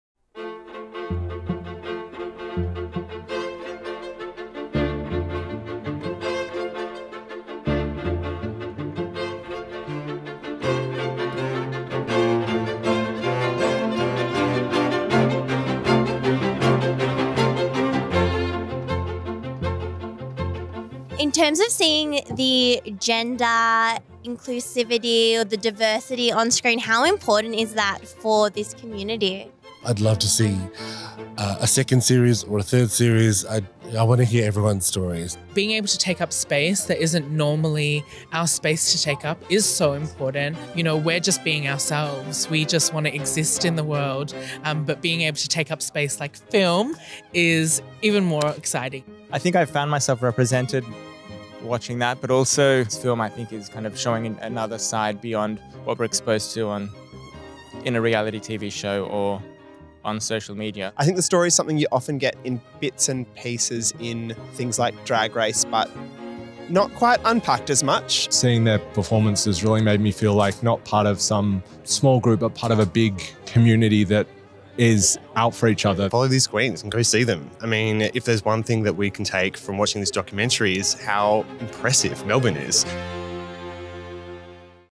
VICTORIAN QUEENS DOCUMENTARY | VIC Pride Centre BTS | Aysha Buffet and Lady Diamond | Cox & Gardner
Behind the scenes footage at the Vic Pride Centre rooftop screening for Melbourne Queer Film Festival.

Victorian Queens explores the significance of Naarm/Melbourne's queer geographies and the Queens who rule them. Starring Lady Diamond, Aysha Buffet, Zodiac & Art Simone.